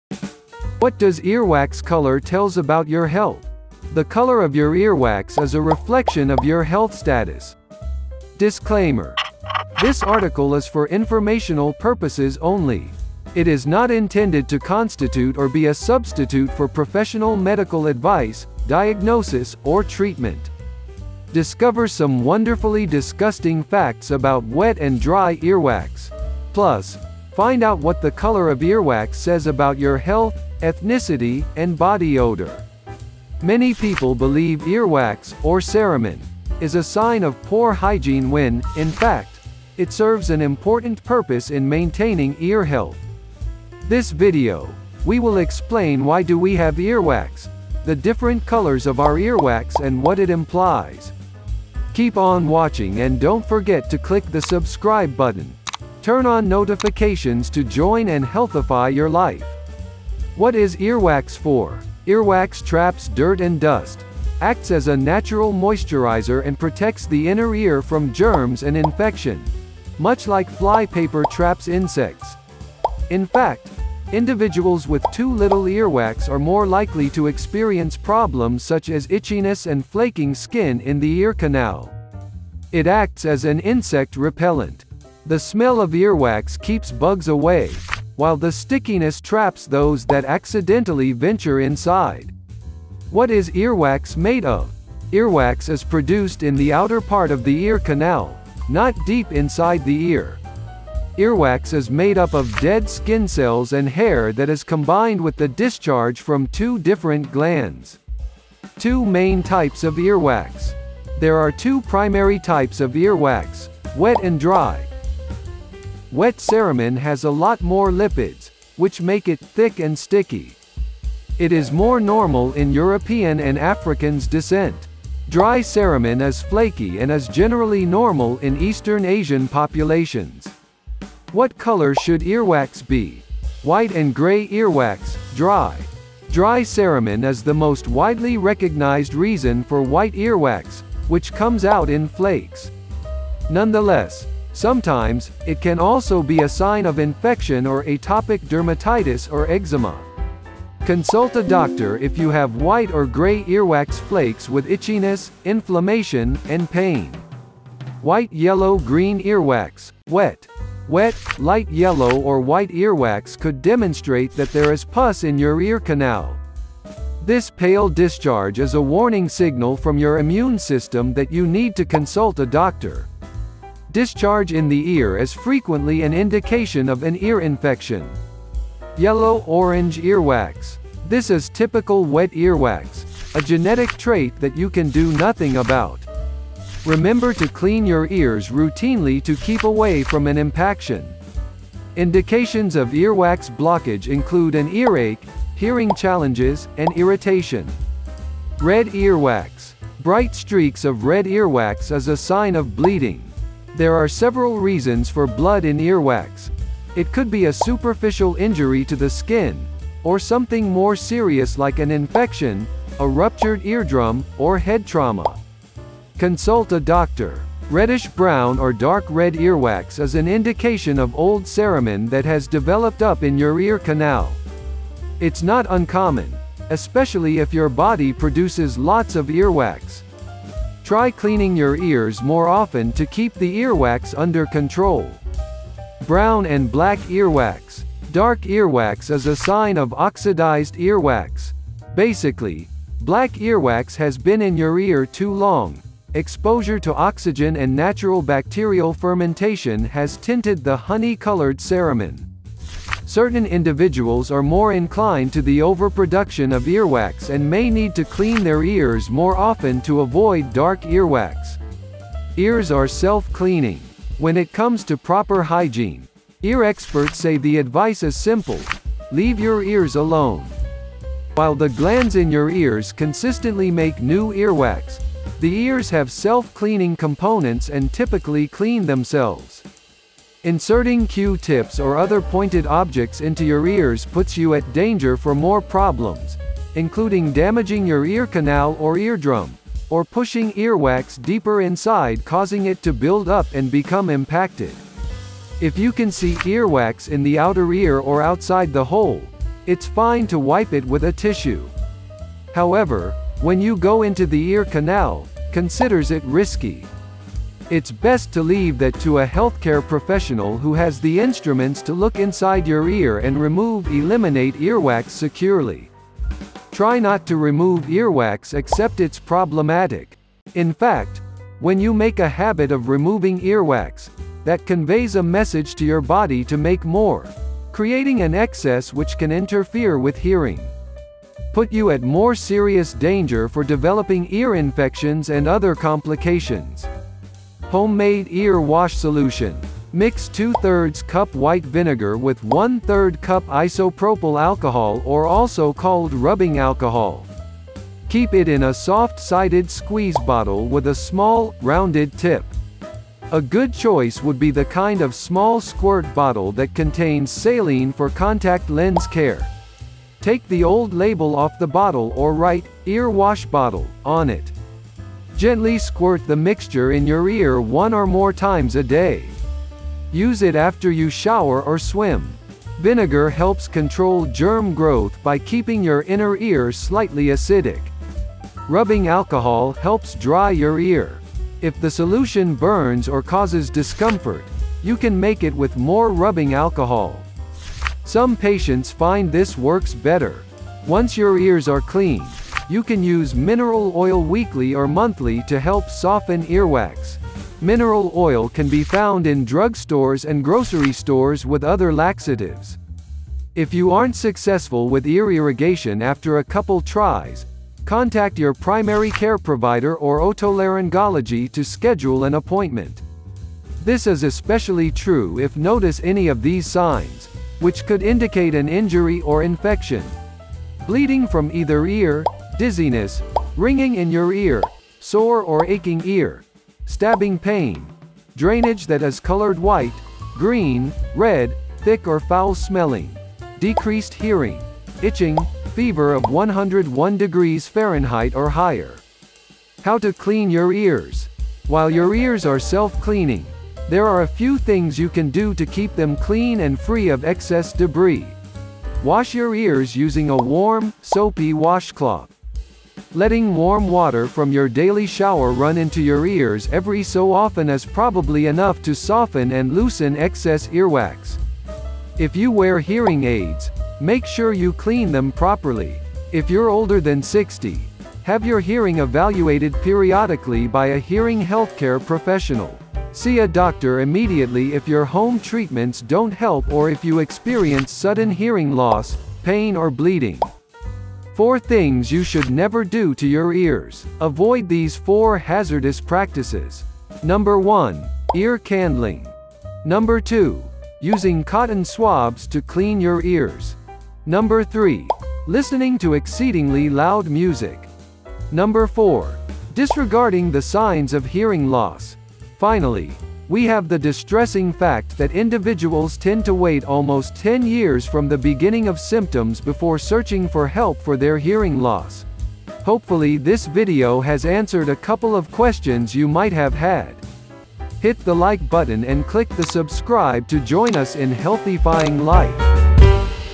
What Colors Of Earwax Says About Your Health
What Does Your Earwax Tells About Your Health

In this eye-opening video, we delve into the fascinating world of earwax and its surprising connection to your overall health. Did you know that the color, texture, and smell of your earwax can reveal essential clues about your well-being? From subtle signs to potential red flags, we uncover the secrets that your earwax holds and what it might be telling you about your health.

Join us as we explore the intriguing science behind earwax analysis, backed by medical experts and research. Understanding these signals can help you take better care of your ears and possibly detect underlying health issues early on. Whether you have dry, flaky earwax or the stickier variety, there's a lot more to it than meets the eye.

Don't miss this enlightening journey as we uncover the mysteries of earwax and its role as a unique health indicator. Let's embark on this earwax adventure together!